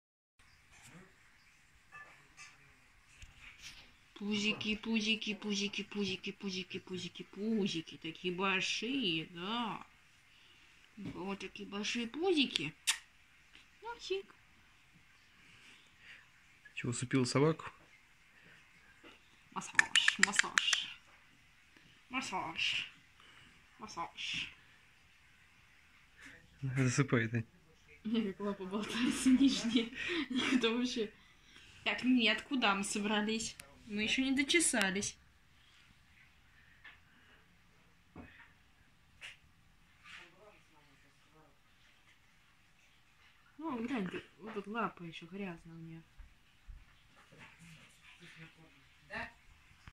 Аня усыпила собаку)))
Малышка 4-х месячная балдеет от массажа!
Это последний щеночек, оставшийся от десяти рождённых Умкой щенят!
Но вот и её уже забрали новые хозяева.
Это последнее видео со Звёздочкой